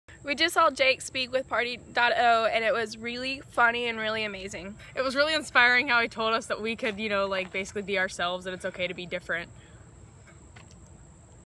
Student Testimonial for Party.0 College Speaker Jake White
Jake White spoke at Erskine College to 350 students about having fun without drugs and alcohol. They loved the interactive presentation and stories about starting a movement around partying sober. Jake also stayed on campus to show them how to host their own events without any drugs or alcohol and they started a chapter of Party.0!

For more information about bringing this message to your students,